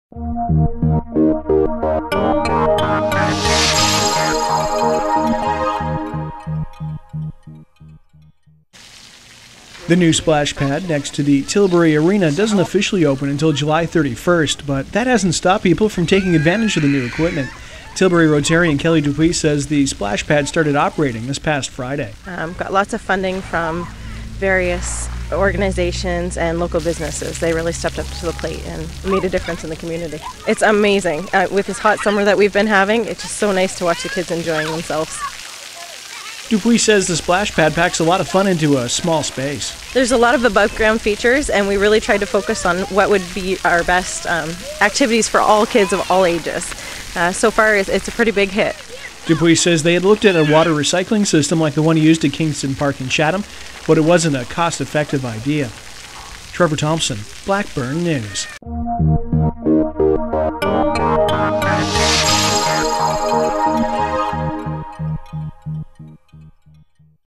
Tilbury Splash Pad Opens
Tilbury splash pad unofficially opened this past Friday.
Video by Trevor Thompson, Blackburn News.
CFCO, CKSY, CKUE